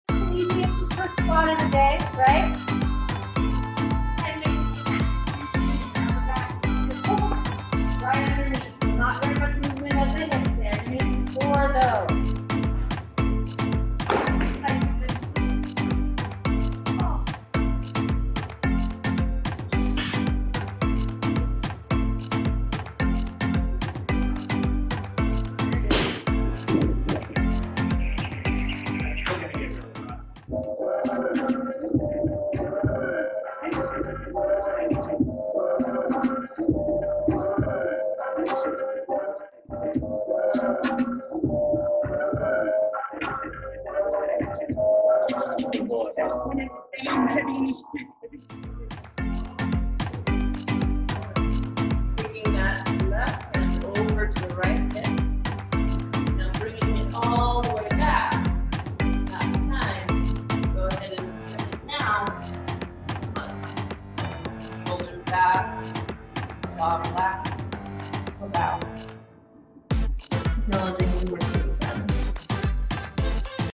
Coach Amy and Yogi Chante provide excellent workout and recovery guidance. Clips from weekend.
Tectonic professionals provide holistic fitness guidance.

Get live oversight from approachable, kind, yet knowledgeable coaches.